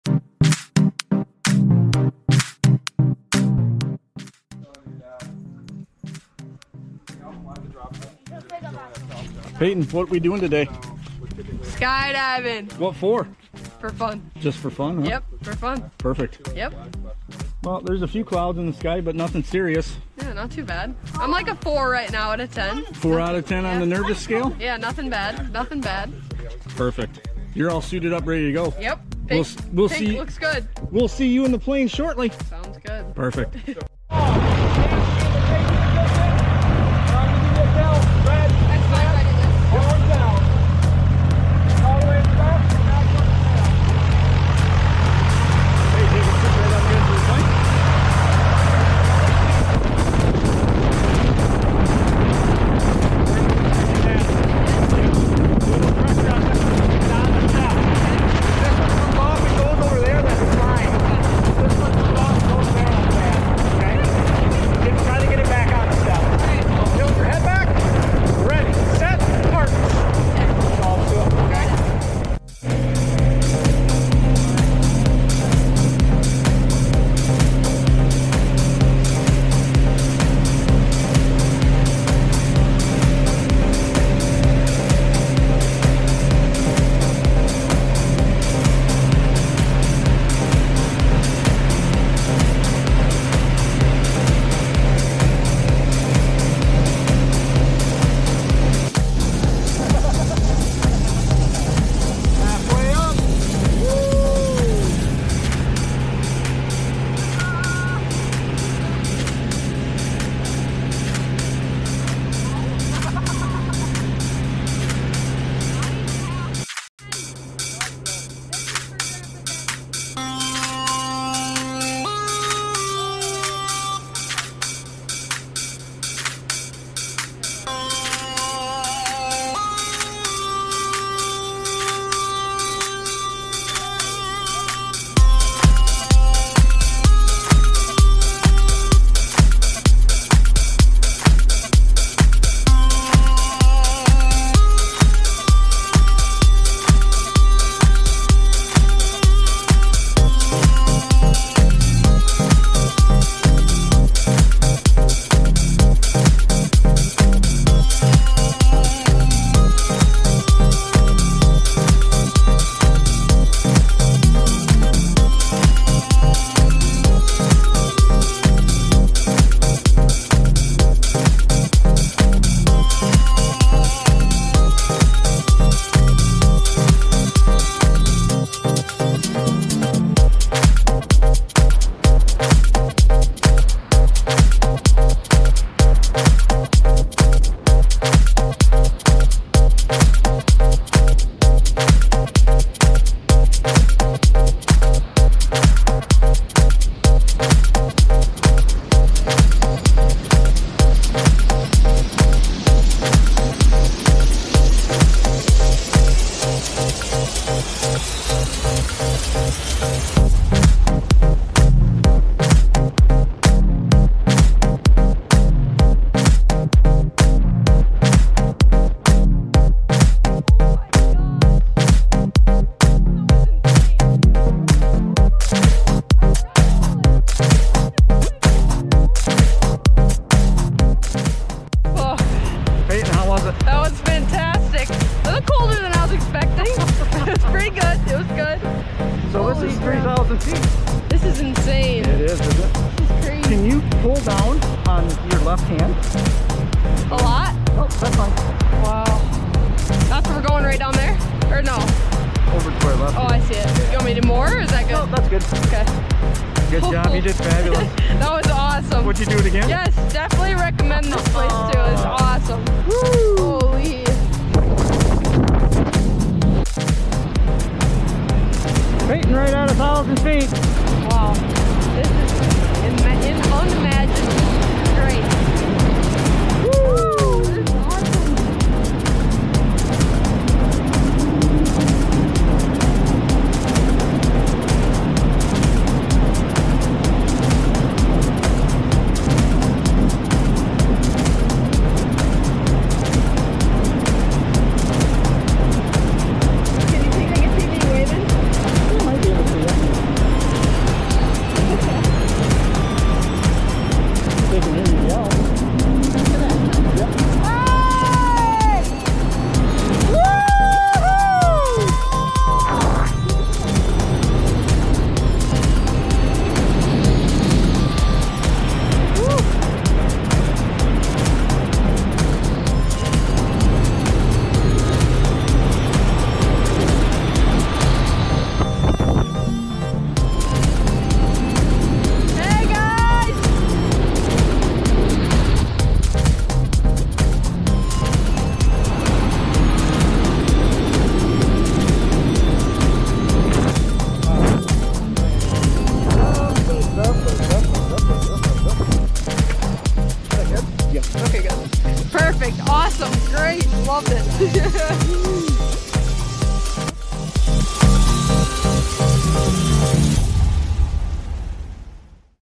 Peyton Skydive
Lake Wissota Skydiving
Chippewa Falls, WI

With Jordan, Jeff (Dad), and Alyssa